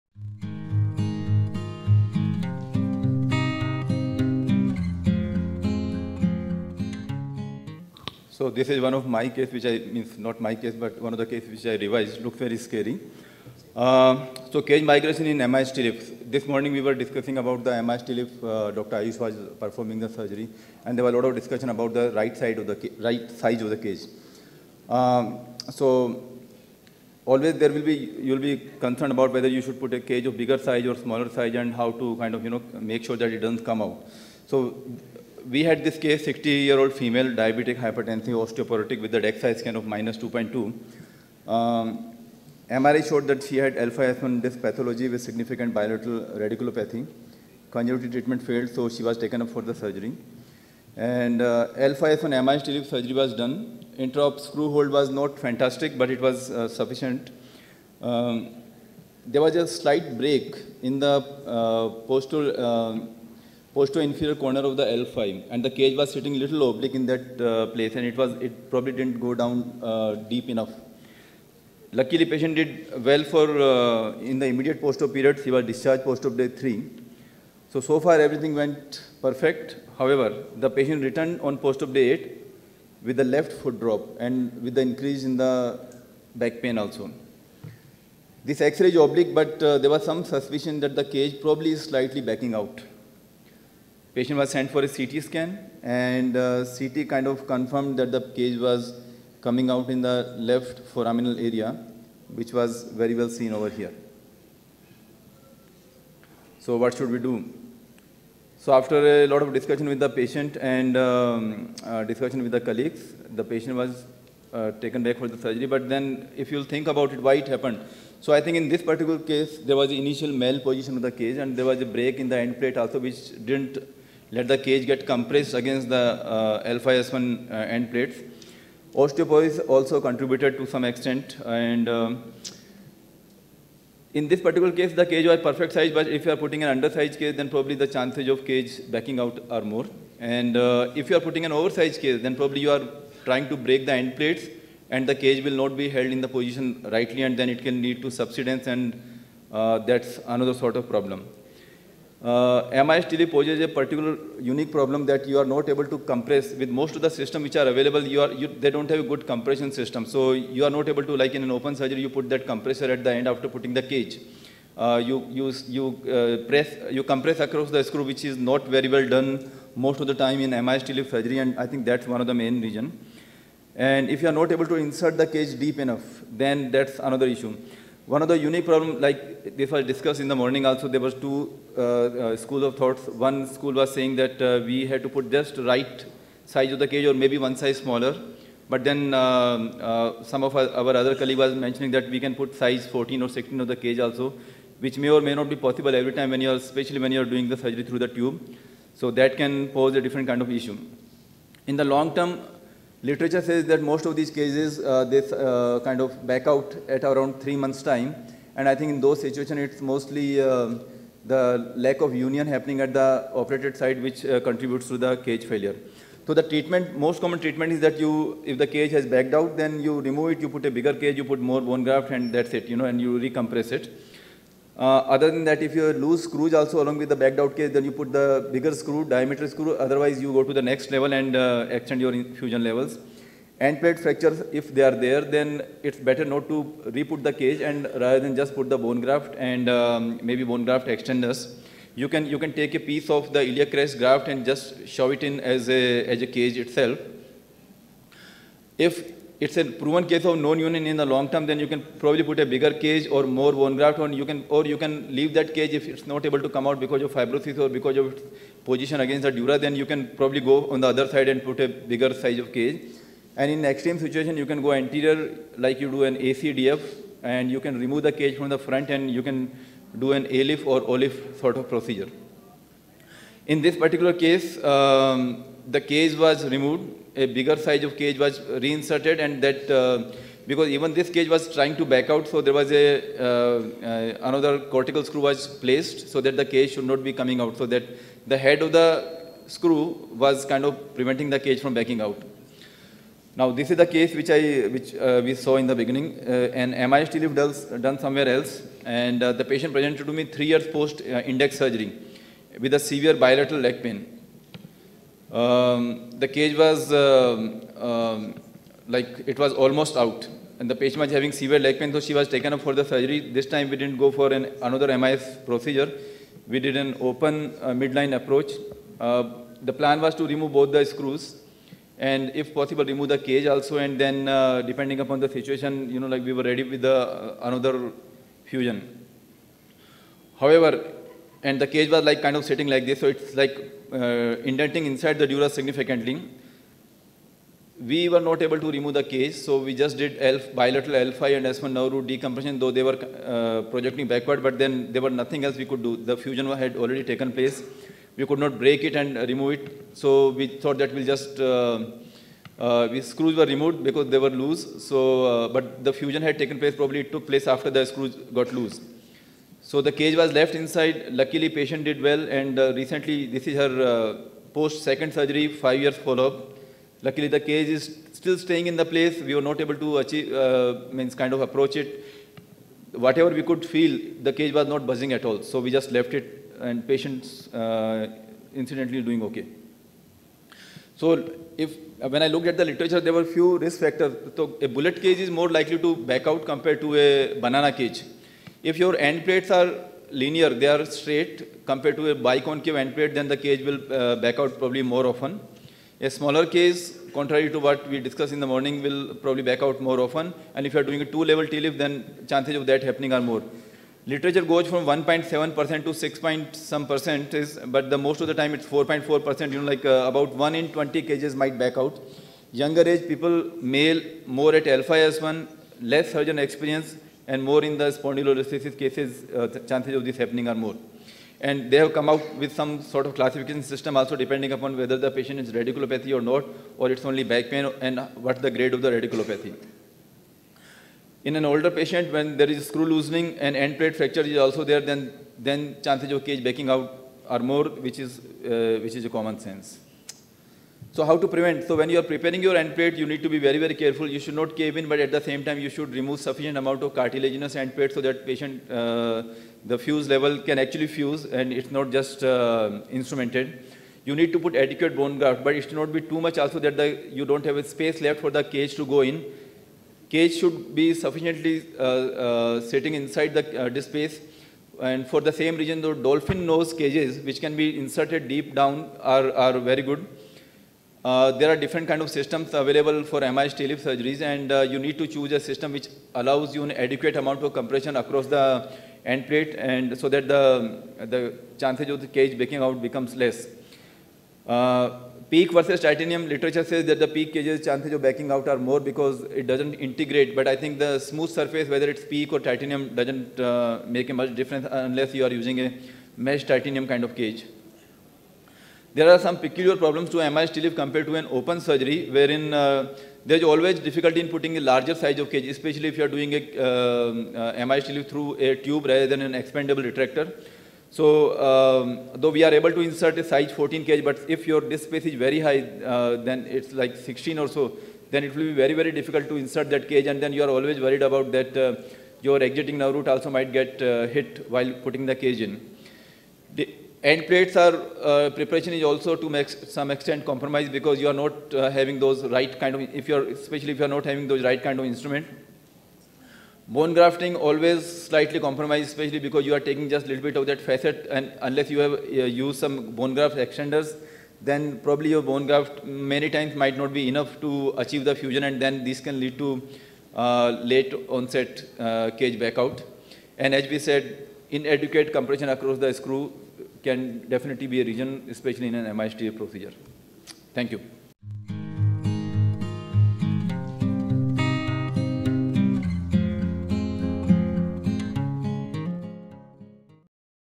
Cage Migration in MIS TLIF - Dr Amit Sharma
This event is part of the Annual Conference of Minimally Invasive Spine Surgeons of Bharath - MISSABSCON 2022, PUNE, INDIA.